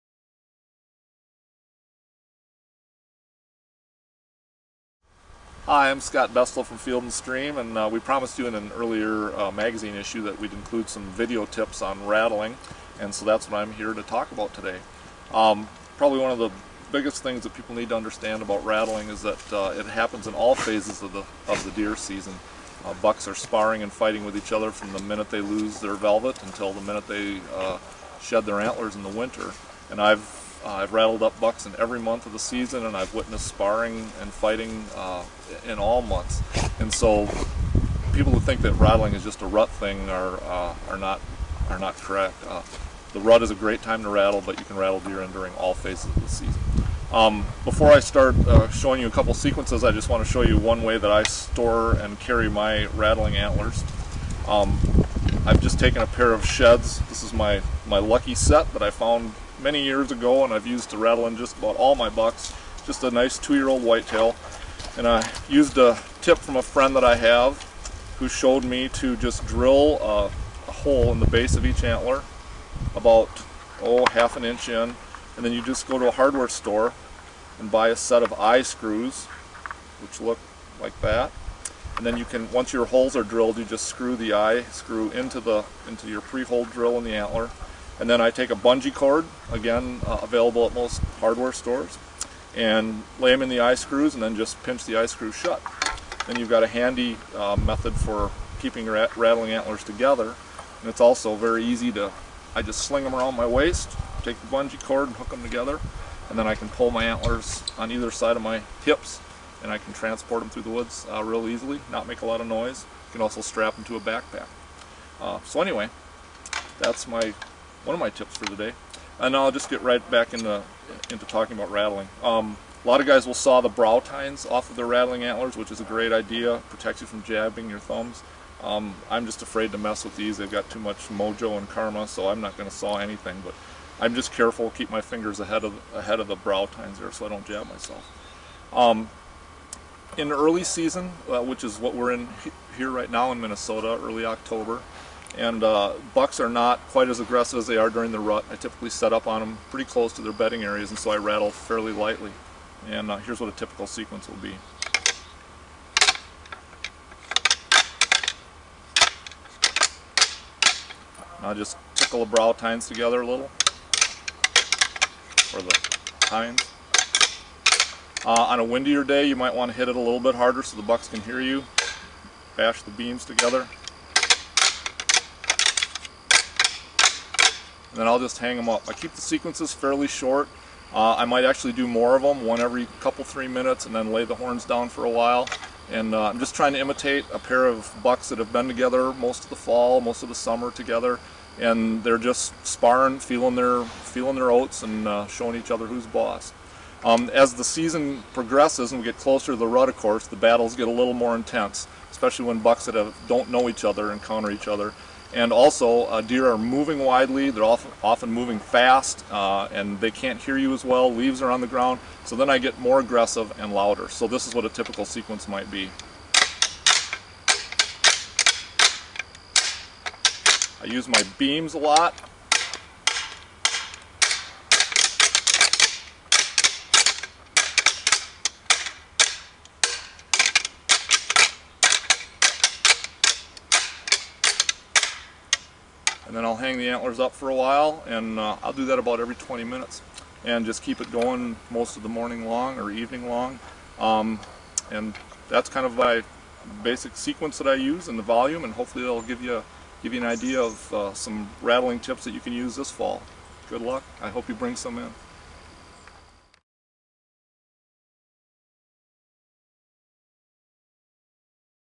Whitetail Rattling Tips from Field & Stream's Scott Bestul
Field & Stream's whitetail deer expert shows you his method for rattling
in big whitetail bucks. See more tips from Scott at whitetail365, his
blog on fieldandstream.com.